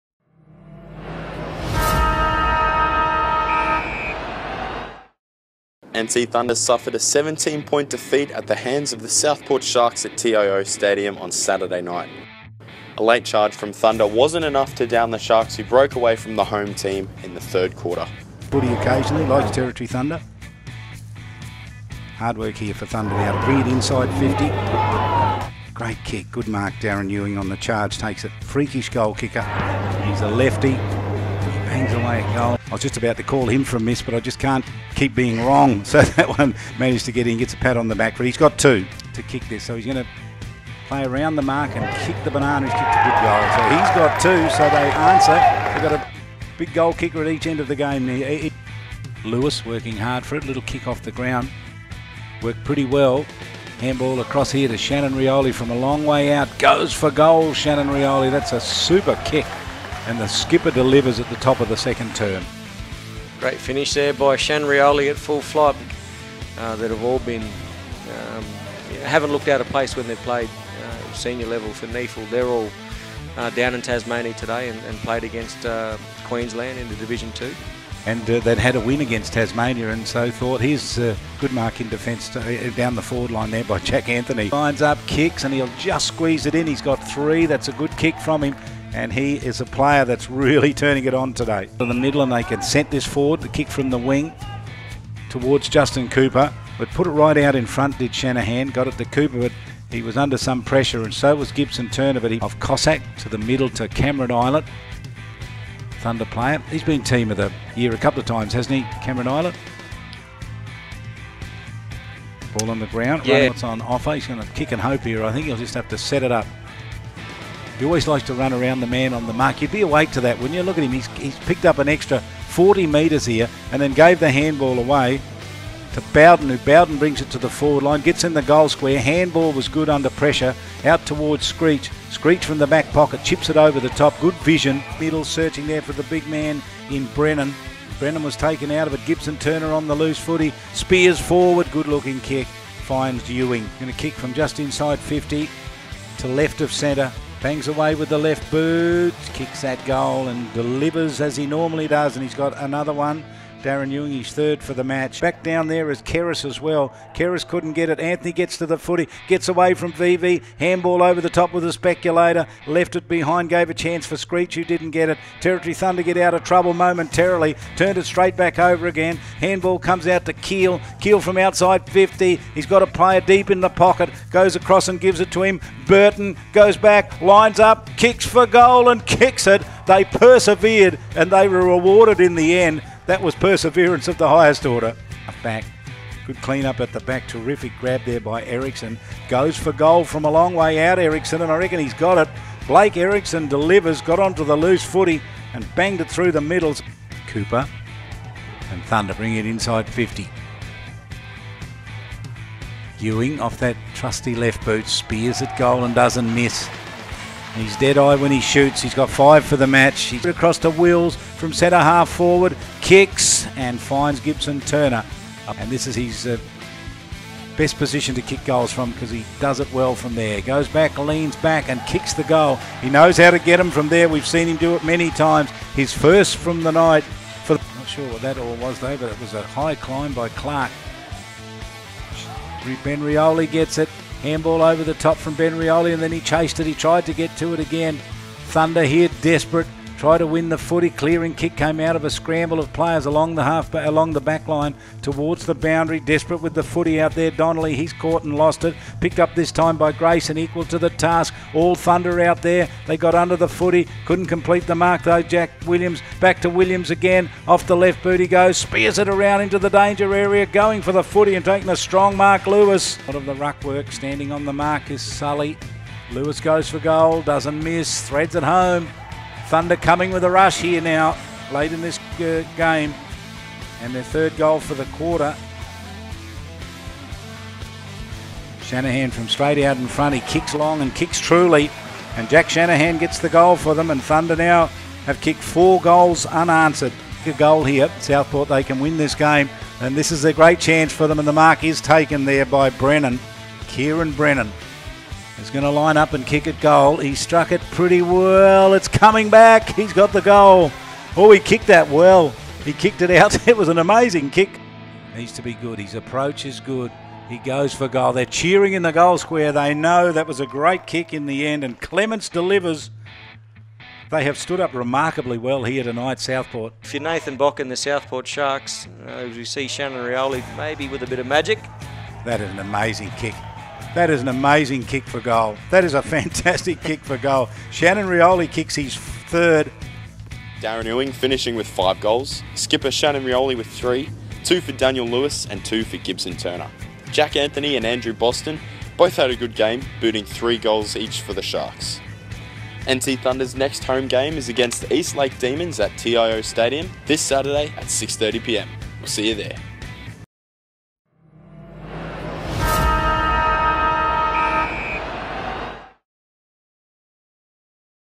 Nt Thunder v Southport Sharks RND 7 Highlights
Thunder played Southport at TIO on Saturday night and went down by 17 points despite a final push that wasn't enough to secure a home team victory.